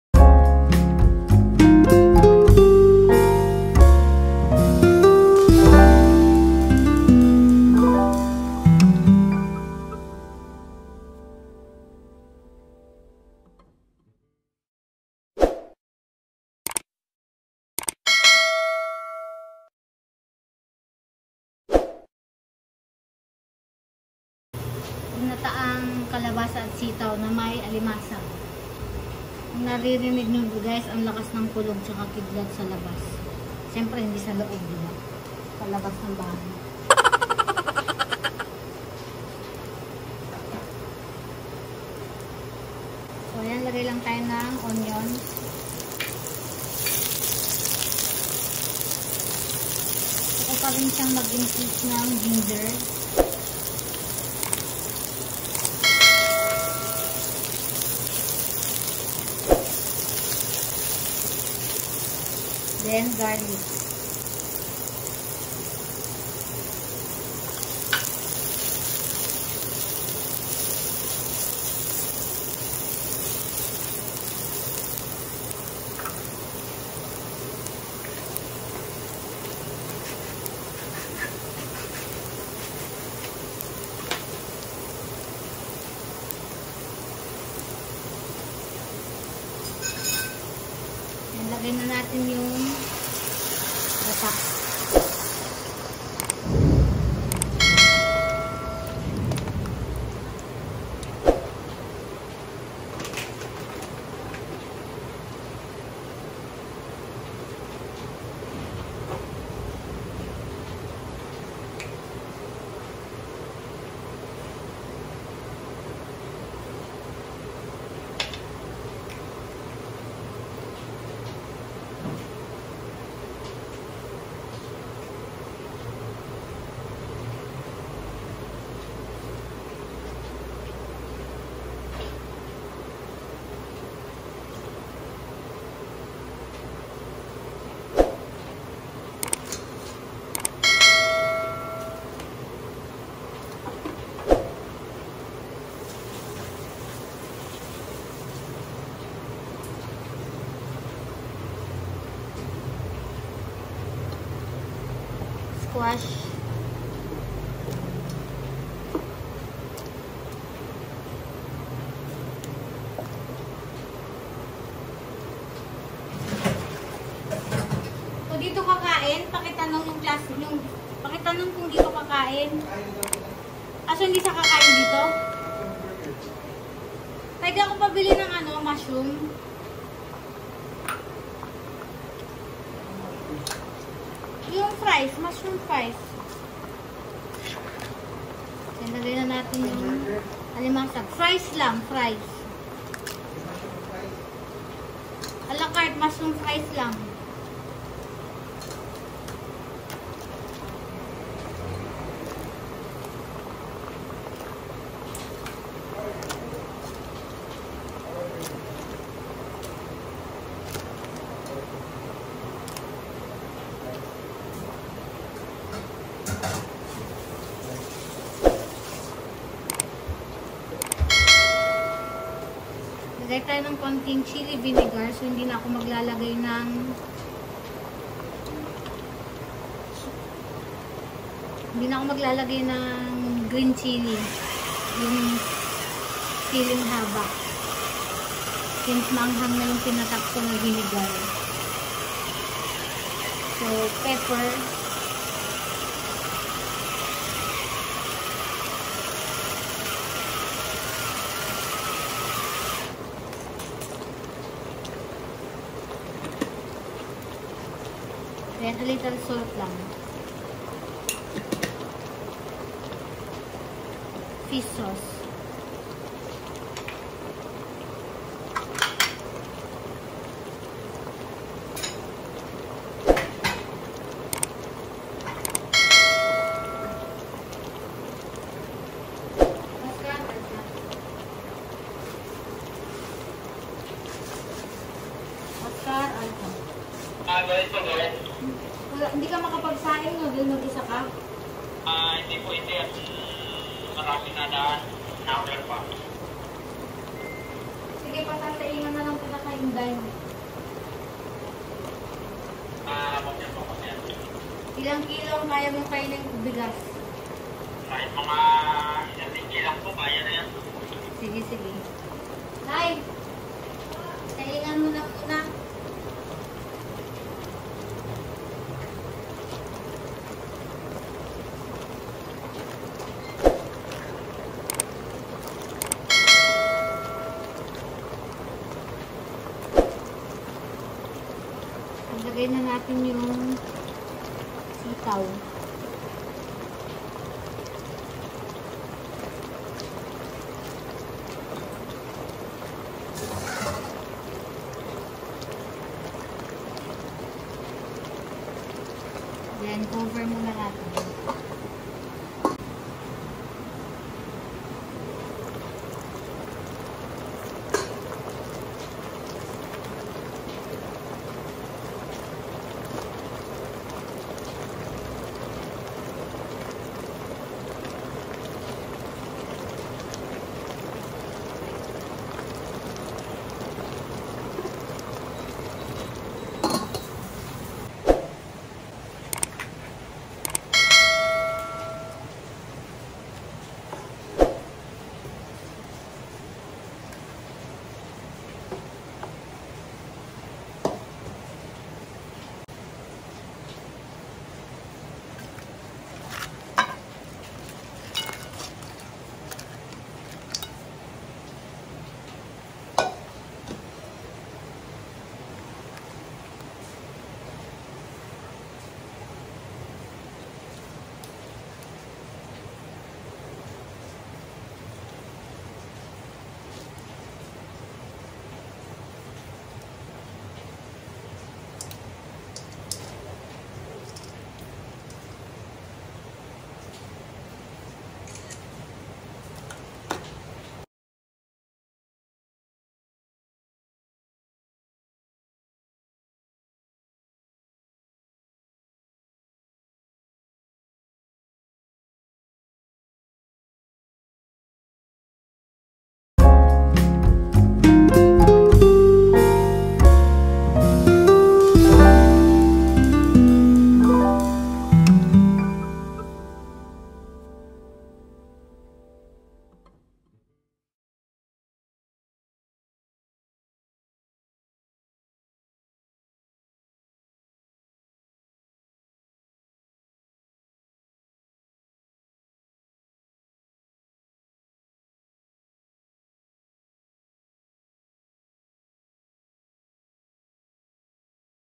Alimasag | Ang lakas ng Ulan!!! | Pero tuloy ang pagluluto |V💙The Great Little Housewife
A delightful dish that captures the essence of coastal flavors and rich culinary traditions. This savory crab dish, cooked in creamy coconut milk, is an irresistible combination of tender crab meat, aromatic spices, and fresh vegetables. Perfect for special occasions or a comforting family meal, Ginataang Alimasag brings a touch of tropical paradise right to your table